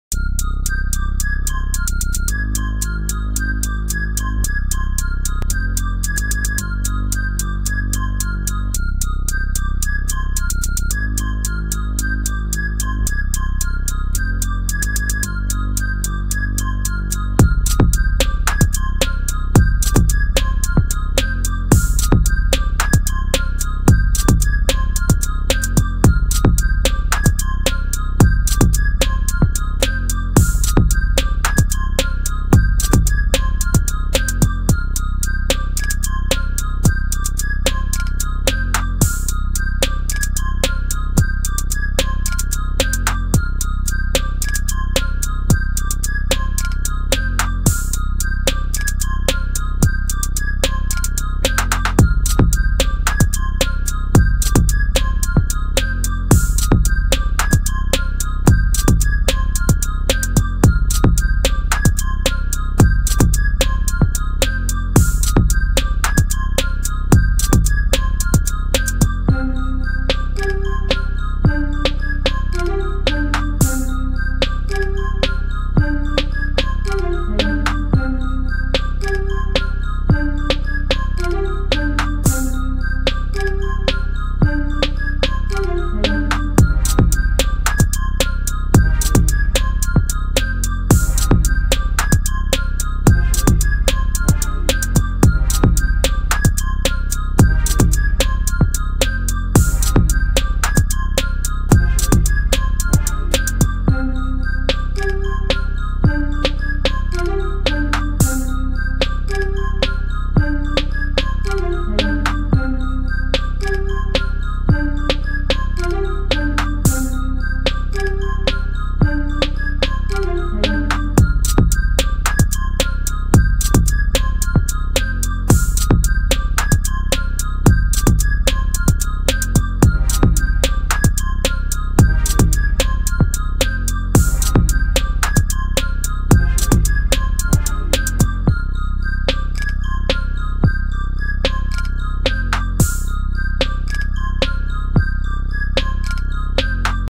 Dark wavy trap beat 111bpm
First beat i made with Serato studio.